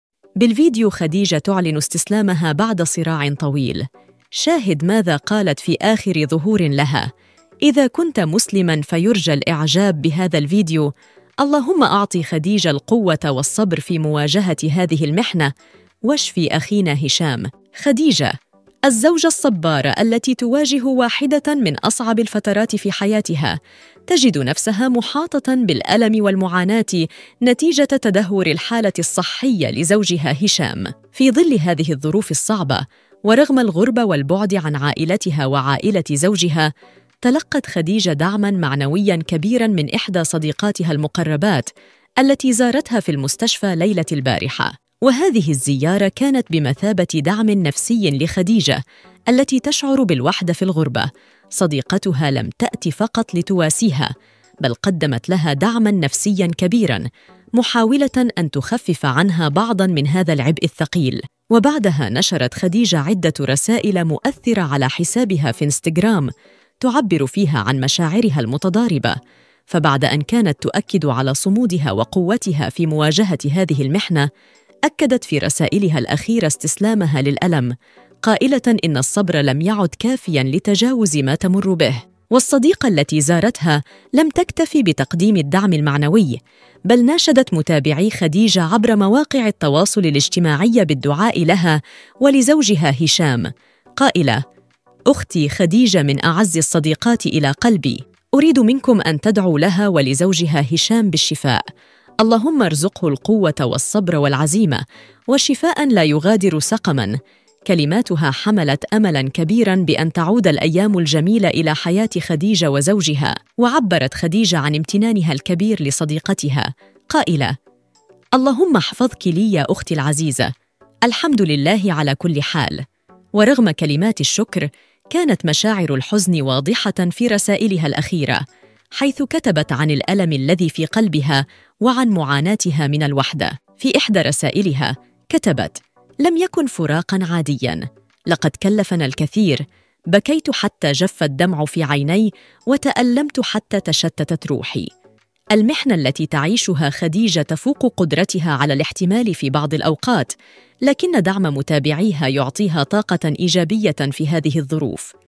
بالفيديو خديجة زوجة هشام تعلن استسلامها هل بُعد العائلة وهي في الغربة وحيدة شاهد ماذا قالت khadija
بالفيديو خديجة زوجة هشام تعلن استسلامها هل بُعد العائلة وهي في الغربة وحيدة شاهد ماذا قالت khadija hicham

هشام وخديجة
خديجة وهشام